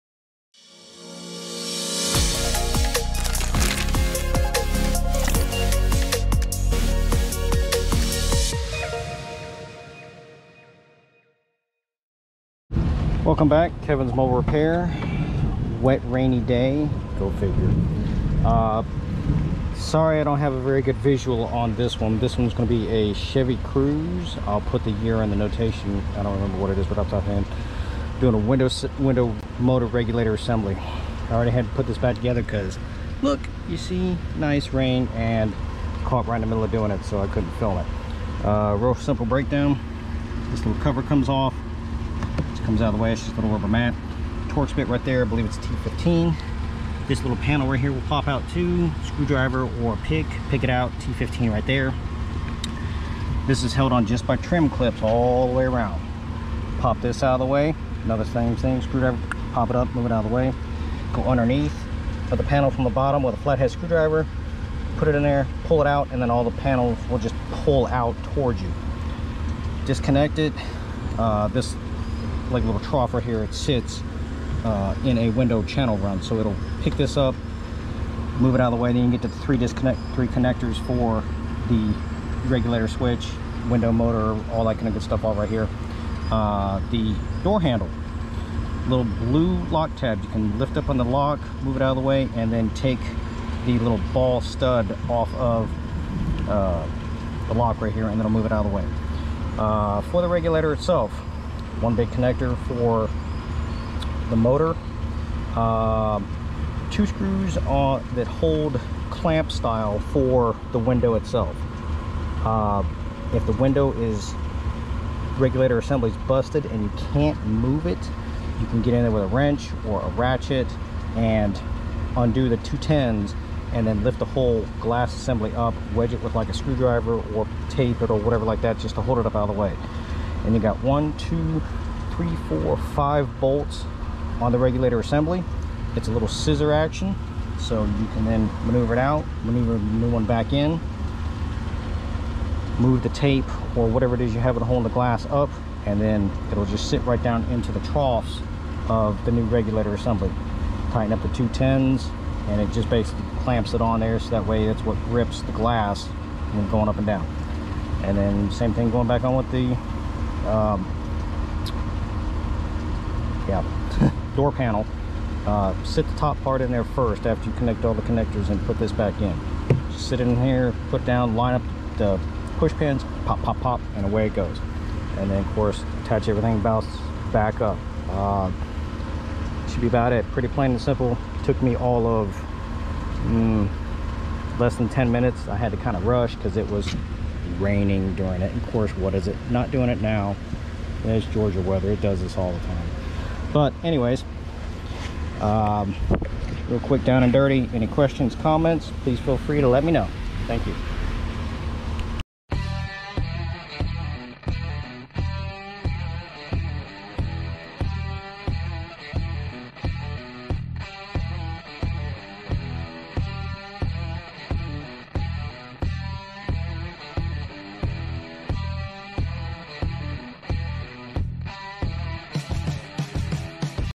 How to replace 2016 Chevrolet Cruise Driver Front Window Regulator Assembly
with a 27 year Master Mechanic. 2016 Chevrolet Cruise Driver Front Window Regulator Assembly